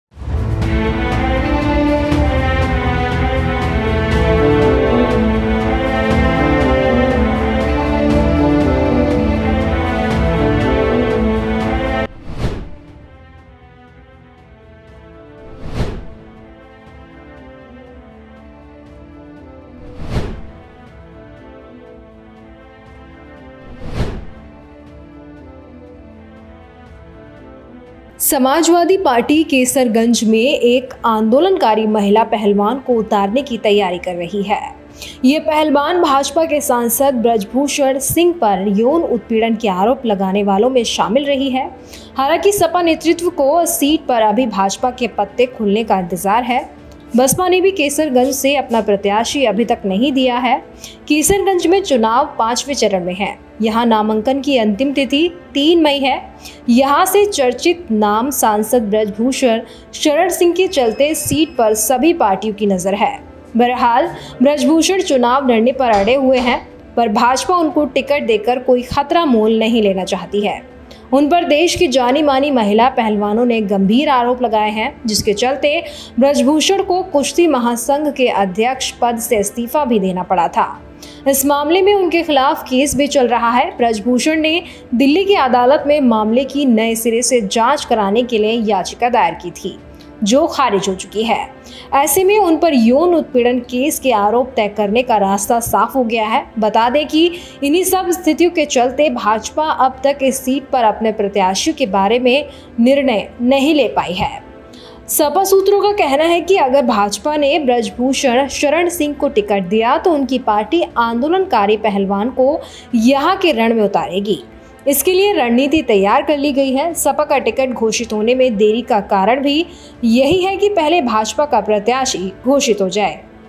सपा क्यों नहीं चल रही है कैसरगंज में कोई दांव | KESARGANJ | BSP | BJP
Samajwadi Party is preparing to field an agitating female wrestler from Kaiserganj. This wrestler has been among those who have accused BJP MP Brij Bhushan Singh of sexual harassment. However, the SP leadership is still waiting for BJP to open its cards on this seat. BSP has also not yet given its candidate from Kaiserganj.